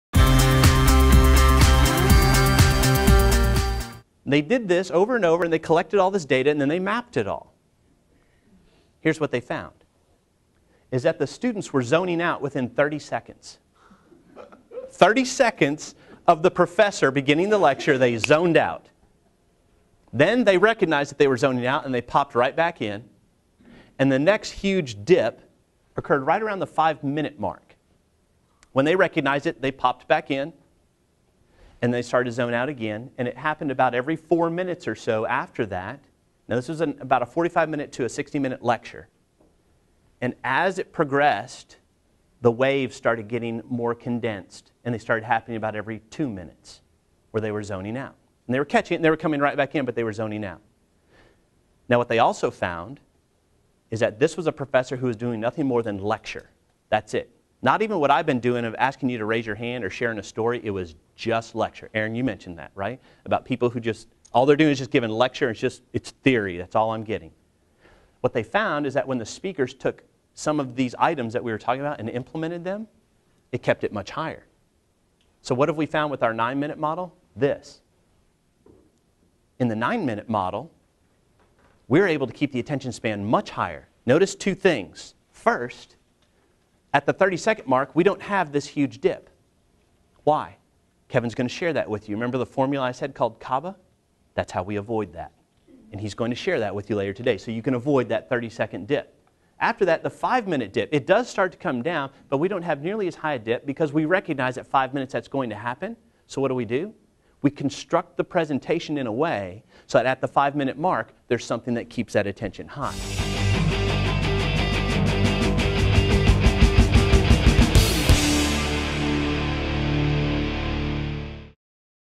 9-Minute Model with Russ Peterson Jr.
In their book, Corporate Ovations, Russ Peterson Jr. (Twitter @RussPetersonJr) and Kevin Karschnik reveal the tools which can be utilized at 30 seconds, 5 minutes and 9 minutes to keep audiences engaged for your presentation.  Start a movement at your organization and start delivering 9 minute presentations!